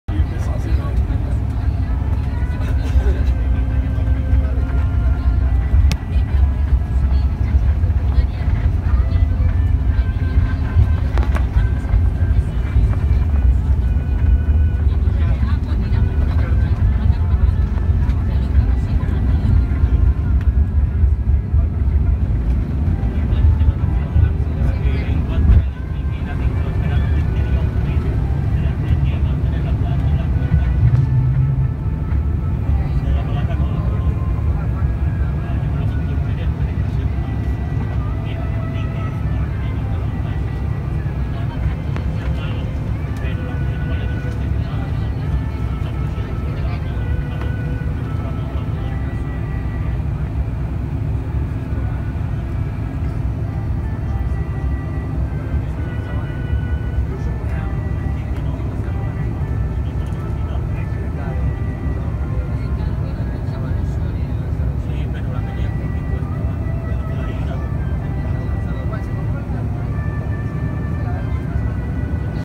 Take off from Tenerife North
Despegando del aeropuerto de Tenerife Norte bajo el cielo cubierto y con el volcán Teide al fondo, tras el techo de nubes. Destino, Gran Canaria.